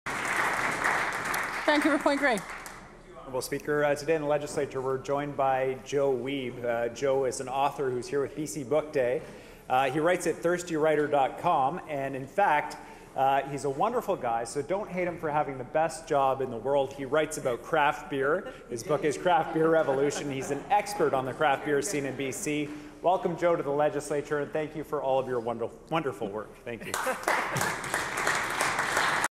Introducing Joe Wiebe in the Legislature.
13 April 2016 - Introducing BC craft beer expert and Craft Beer Revolution author Joe Wiebe in the legislature. Read more of Joe's work at ThirstyWriter.com